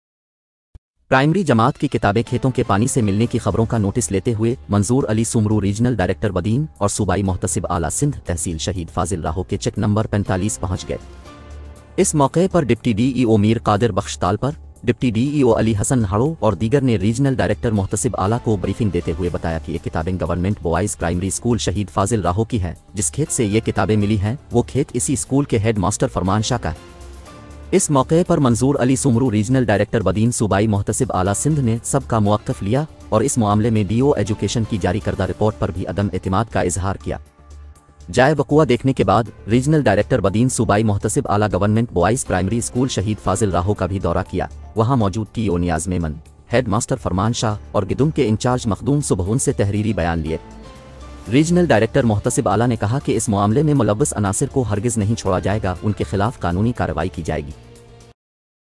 کتابیں کھیتوں ملنے کا معاملہ: ریجنل ڈائریکٹر بدین شہید فاضل راہو کے چک نمبر 45 پہنچ گئے
بدین (  ساون خاصخيلي )
پرائمری جماعت کی کتابیں کھیتوں کے پانی سے ملنے کی خبروں کا نوٹس لیتے ہوئے منظور علی سومرو ریجنل ڈائریکٹر بدین اور  صوبائی محتسب اعلیٰ سندھ تحصیل شہید فاضل راہو کے چک نمبر 45 پہنچ گئے ۔ اس موقعے پر ڈپٹی ڈی ای او میر قادر بخش تالپر، ڈپٹی ڈی ای او علی حسن نہڑیو اور دیگر نے ریجنل ڈائریکٹر محتسب اعلیٰ کو بریفنگ دیتے ہوئے بتایا کہ یہ کتابیں گورنمنٹ بوائز پرائمری اسکول شہید فاضل راہو کی ہیں جس کھیت سے یہ کتابیں ملی ہیں وہ کھیت اسی اسکول کے ہیڈ ماسٹر فرمان شاہ کا ہے۔  اس موقعے پر منظور علی سومرو ریجنل ڈائریکٹر بدین صوبائی محتسب اعلیٰ سندھ نے سب کا مؤقف لیا اور اس معاملے میں ڈی او ایجوکیشن کی جاری کردہ رپورٹ پر بھی عدم اعتماد کا اظہار کیا۔  جائے وقوعہ دیکھنے کے بعد ریجنل ڈائریکٹر بدین صوبائی محتسب اعلیٰ گورنمنٹ بوائز پرائمری اسکول شہید فاضل راہو کا بھی دورہ کیا ، وہاں موجود ٹی ای او نیاز میمن، ہیڈ ماسٹر فرمان شاہ اور گدام کے انچارج مخدوم صبحان سے تحریری بیان لیے۔ ریجنل ڈائریکٹر محتسب اعلیٰ نے کہا کہ اس معاملے میں ملوث عناصر کو ہرگز نہیں چھوڑا جائے گا ان کے خلاف قانونی کارروائی کی جائے گی۔